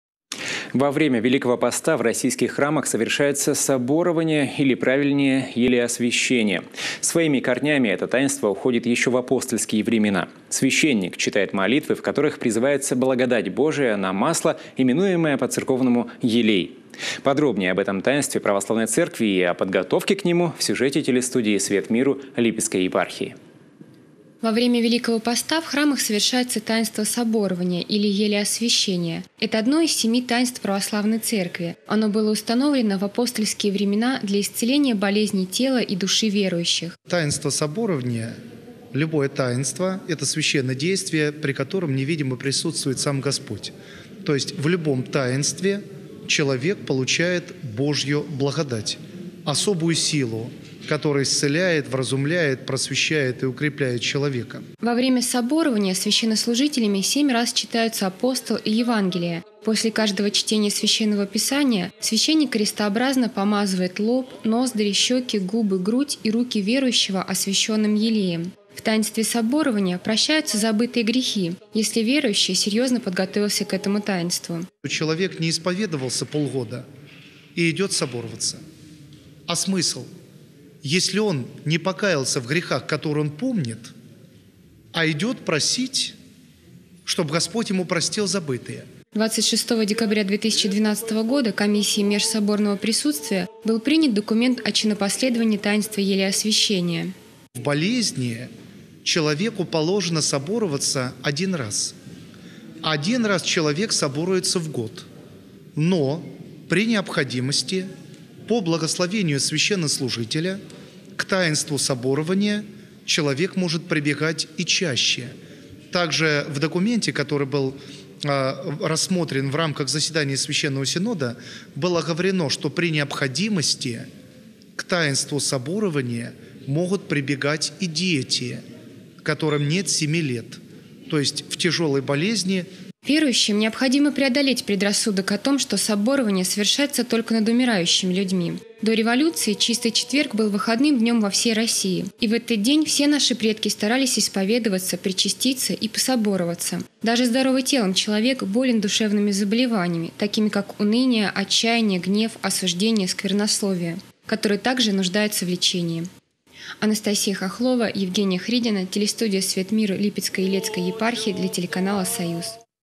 Во время Великого поста в российских храмах сов
Во время Великого поста в российских храмах совершается Соборование или правильнее Елеосвящение. Своими корнями это таинство уходит еще в апостольские времена. Священник читает молитвы, в которых призывается благодать Божия на масло, именуемое по-церковному «елей».